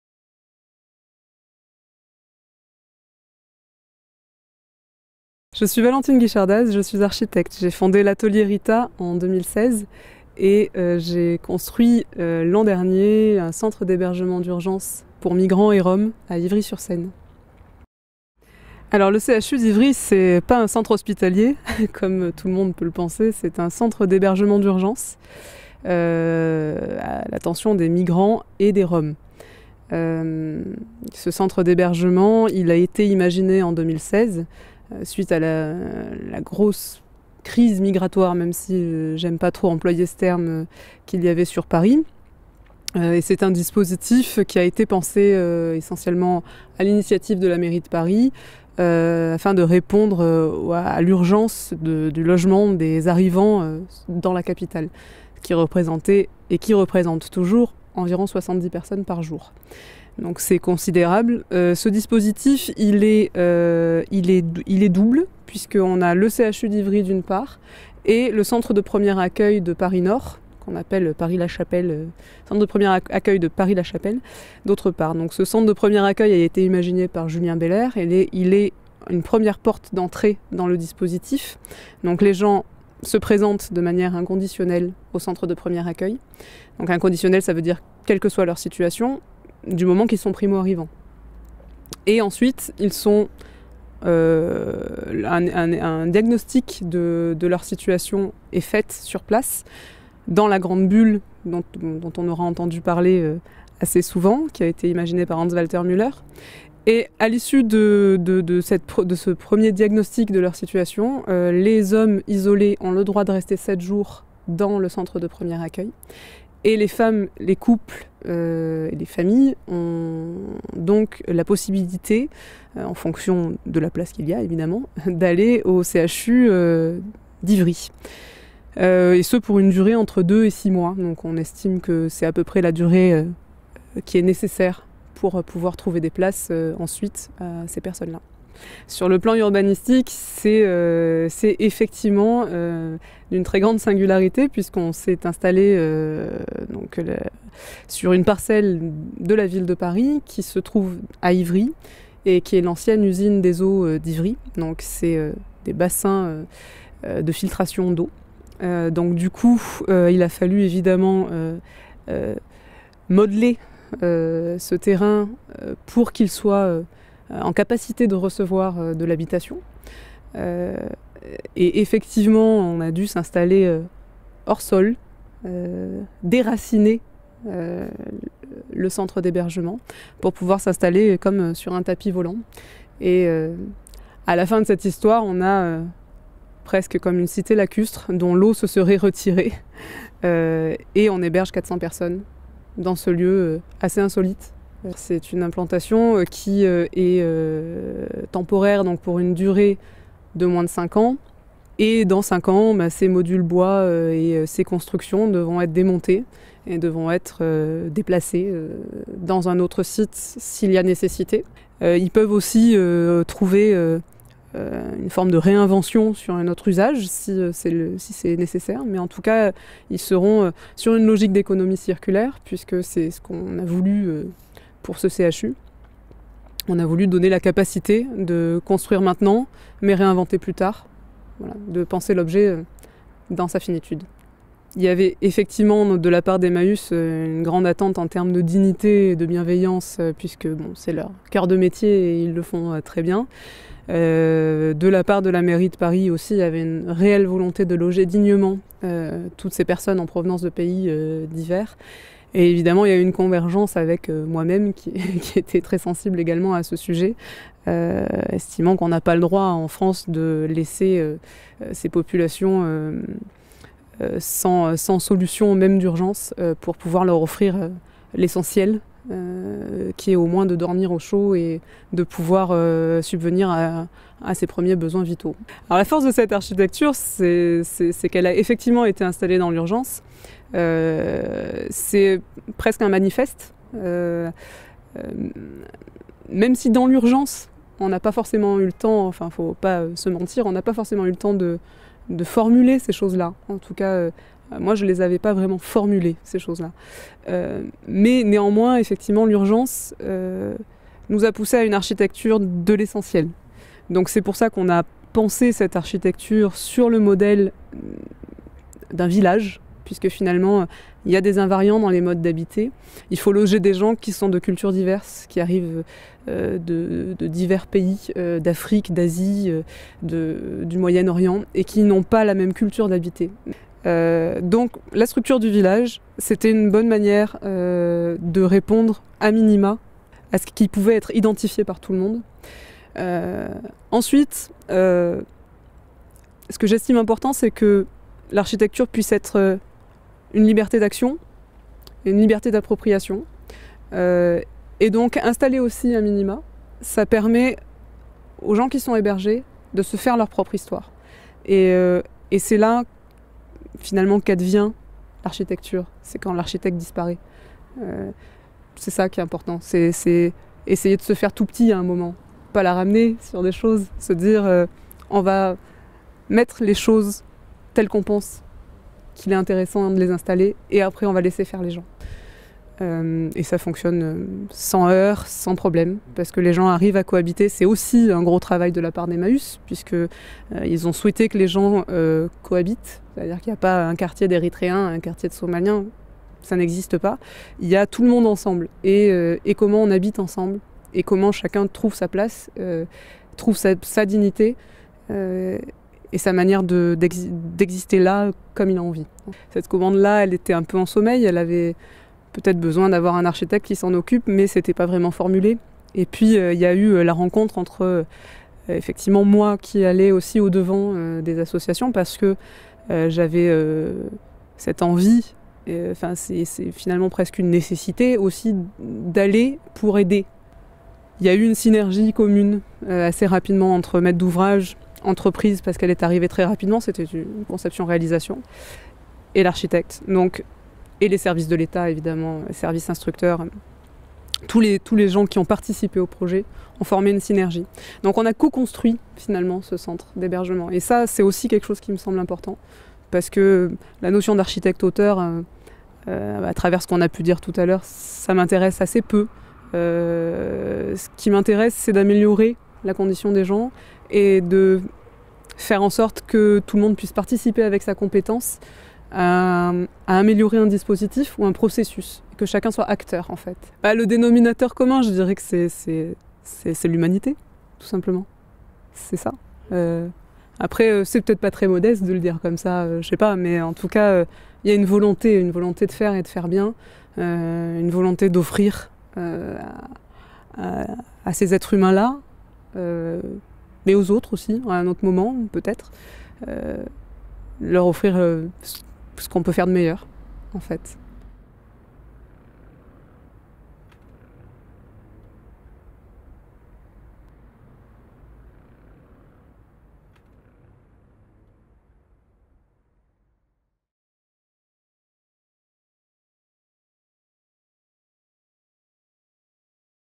interview de Valentine Guichardaz - architecte
Valentine Guichardaz intervenait à Brest le 20 mars 2018 dans le cadre du printemps de l'architecture pour sa réalisation de CHU (Centre d'hébergement d'Urgence) à Ivry-sur-Seine.

Valentine Guichardaz-Versini gère RITA.

Elle est diplômée de l'ENSA-Marseille en 2008 et habilitée à la maitrise d'œuvre en nom propre en 2009. Valentine Guichardaz-Versini est à son compte depuis 2012. Elle a bâti son expérience dans différentes agences à Marseille puis Paris avant de fonder, en 2016, l’atelier RITA.